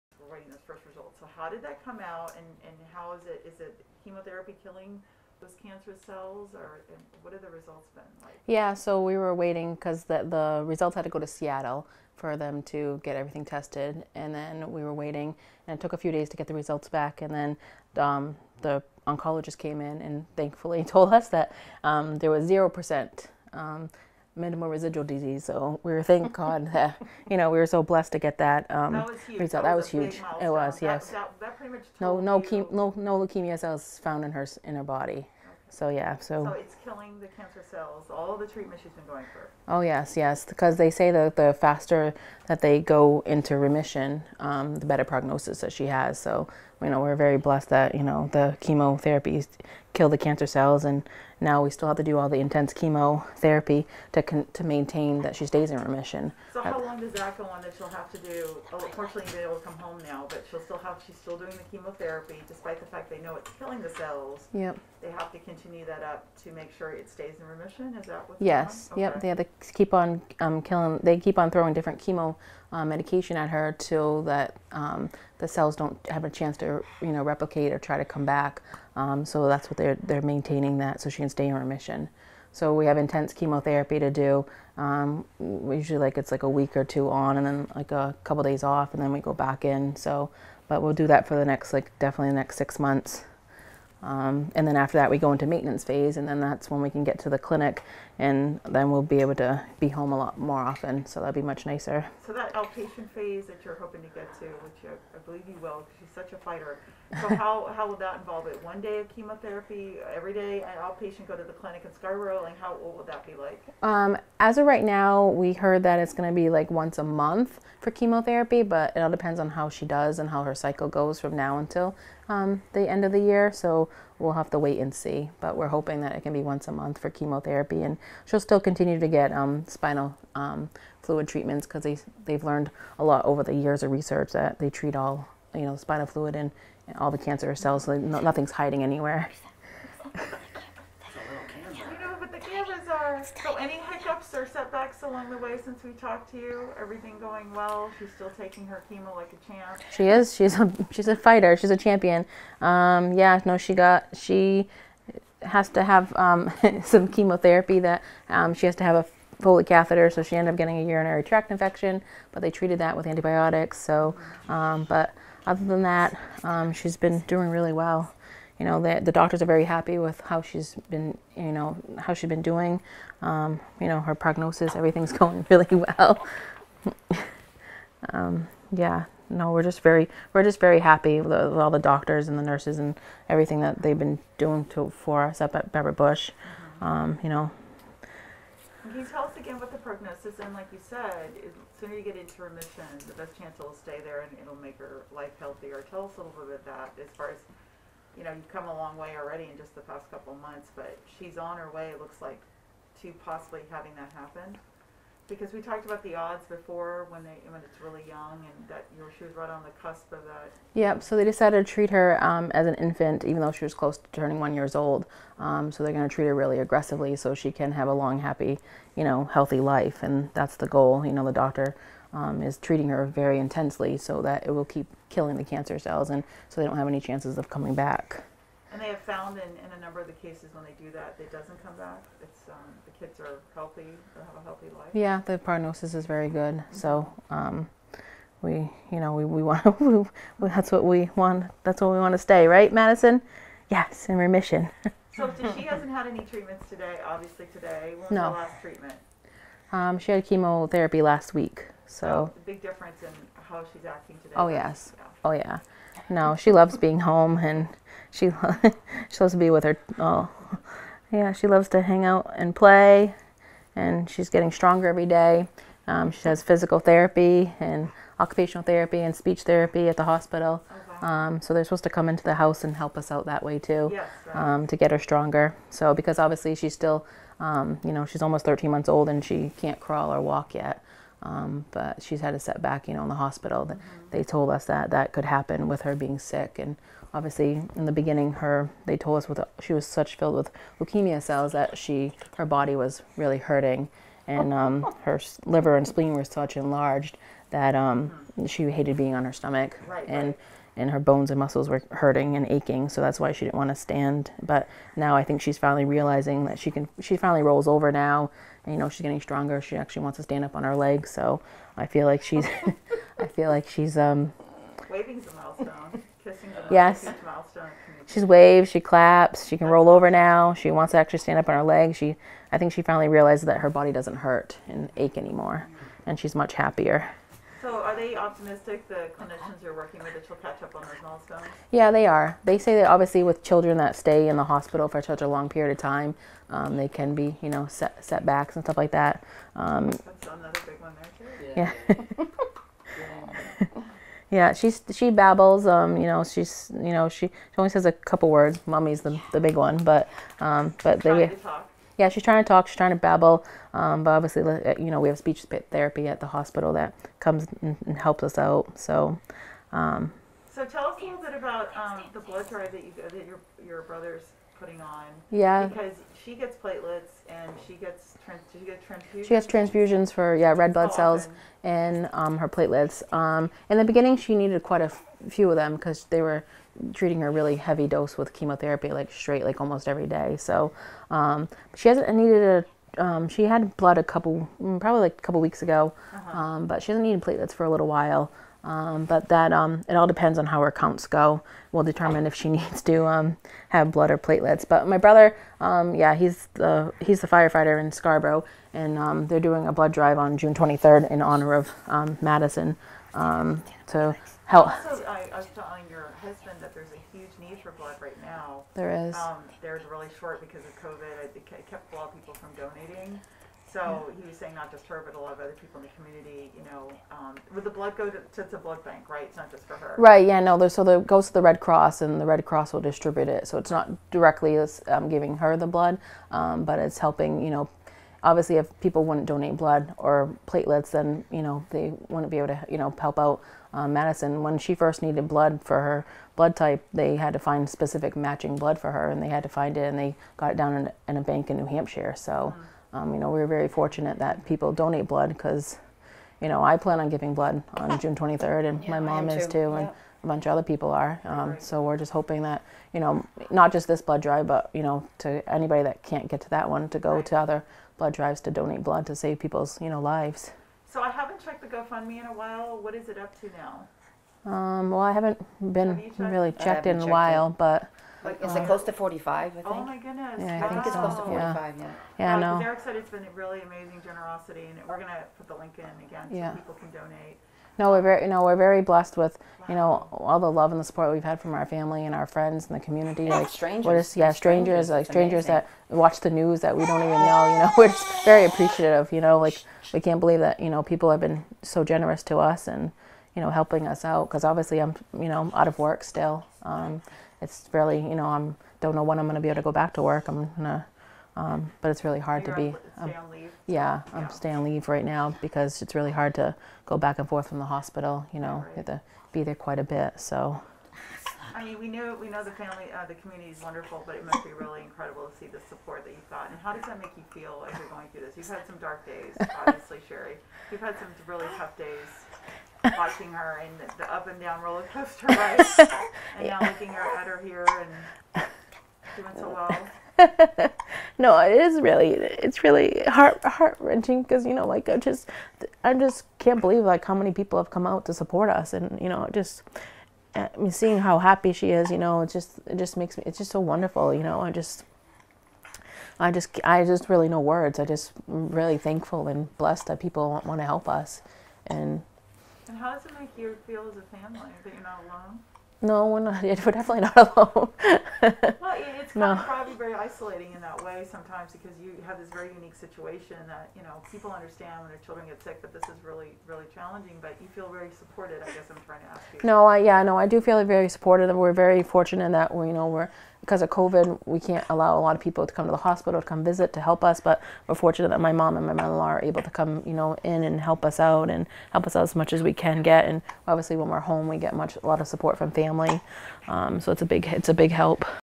Full interview: Maine mother shares updates on her baby daughter's battle with leukemia
The last time we met Madison Charland, the baby girl was fighting for her life after being diagnosed with childhood leukemia.